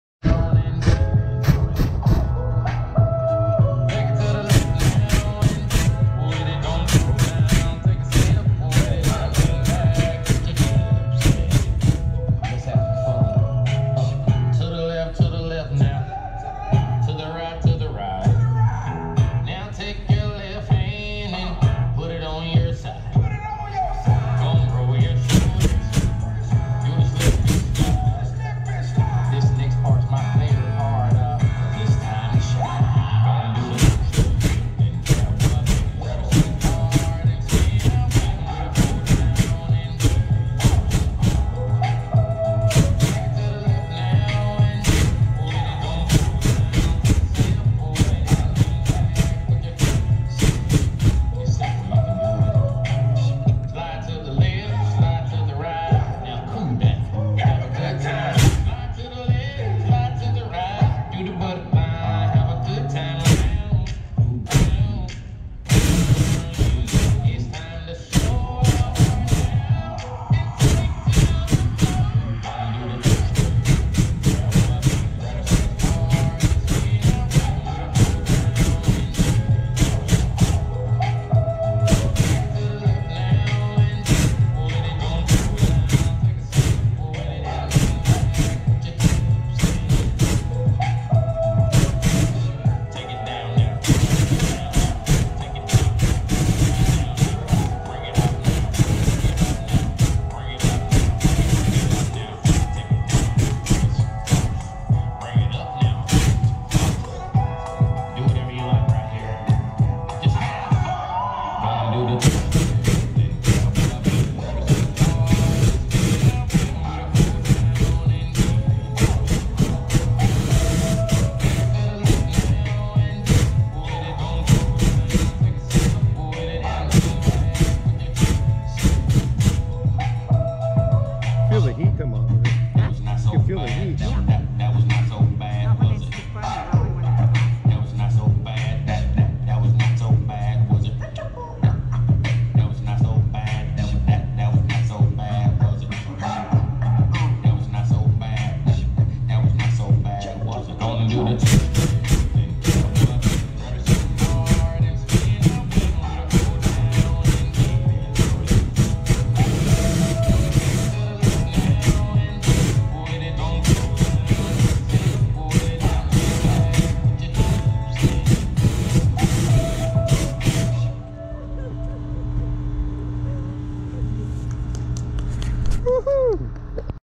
Fire Mantis - Container Park - Las vegas
Fire Mantis in Container Park in Las Vegas, Nevada.